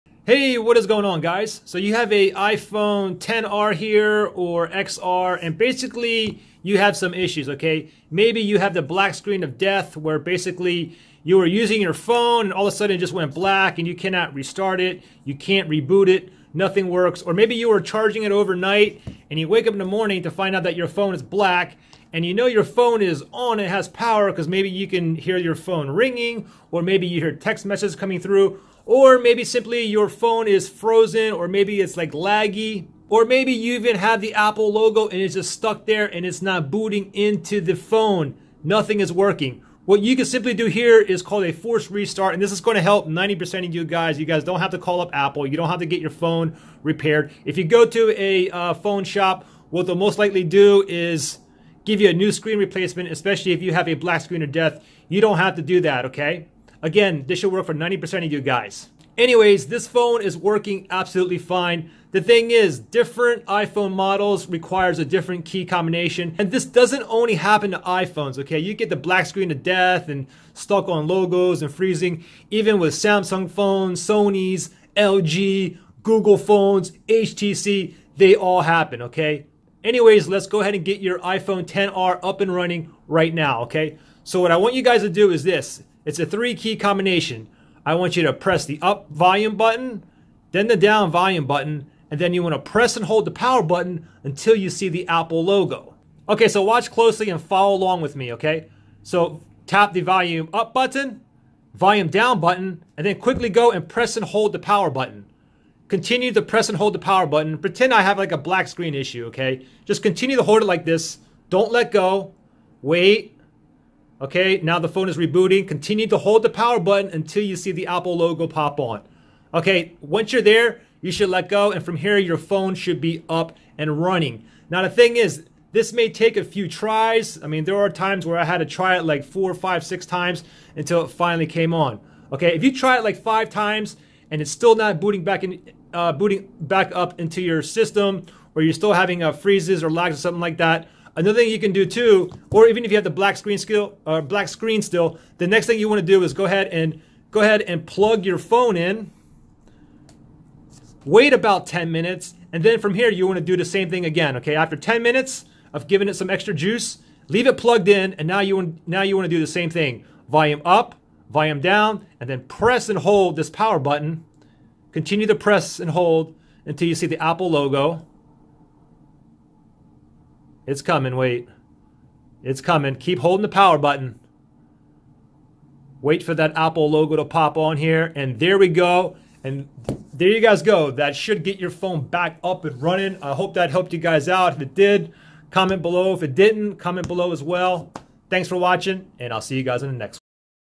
iPhone XR Fixed! Black Screen, Laggy, Frozen, Stuck on Apple Logo (Try this First!)
Note if this video was not helpful then you can try Reiboot

Here is a quick tutorial on how to fix an iPhone XR that has a black screen, frozen, laggy, unresponsive, stuck on apple logo, won't boot or stuck in a boot loop.
Before you take your iPhone to a repair shop or call Apple try this first so you don't waste your time. Remember this happens to all phones not only iPhones.
Anyway what you are doing here is a simple forced restart. Plz like if this worked for you. And subbing would be a bonus thx.